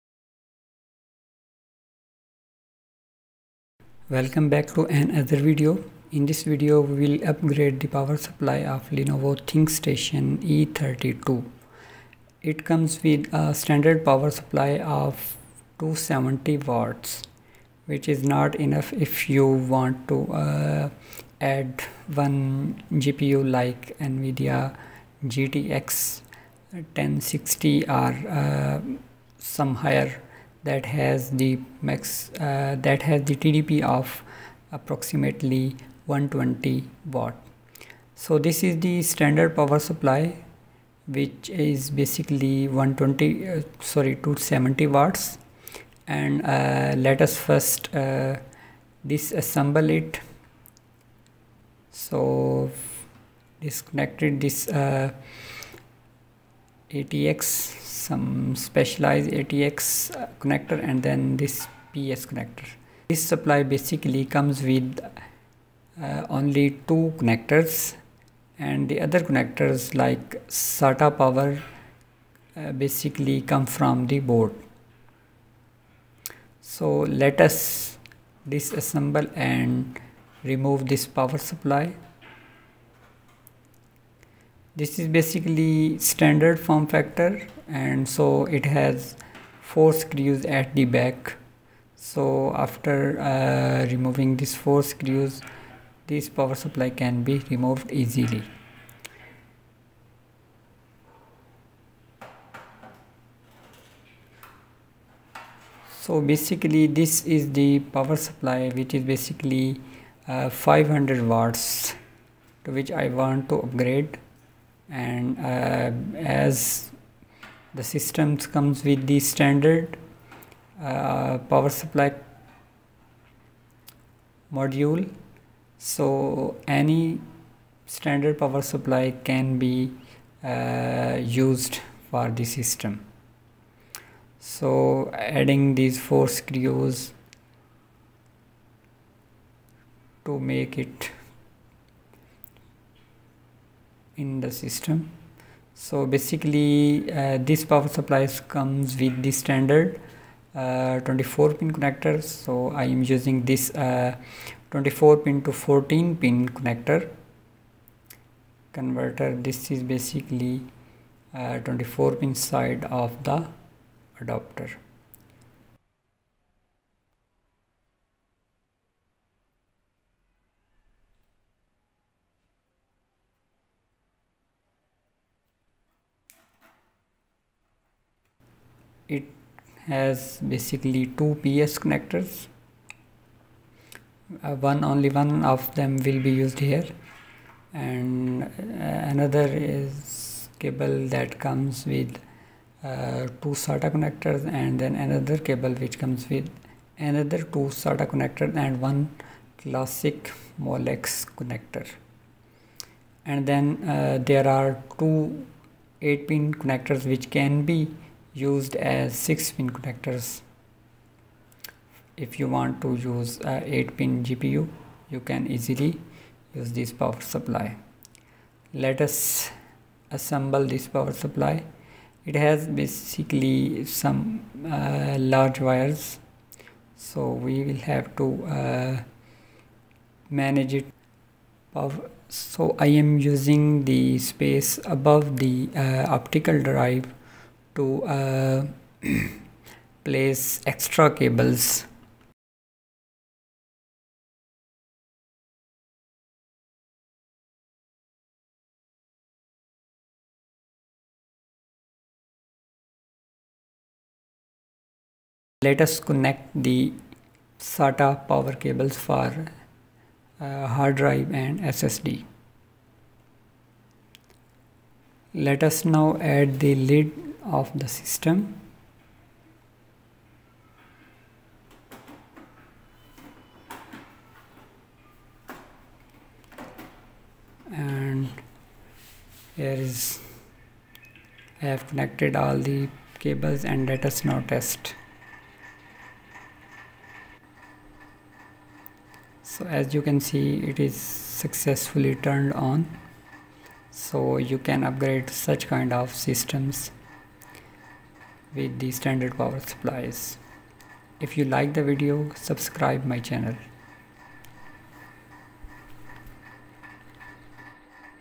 Lenovo Think Station Power Supply Upgrade
In this video It has been demonstrated that how lenovo thinkstation power supply can be upgraded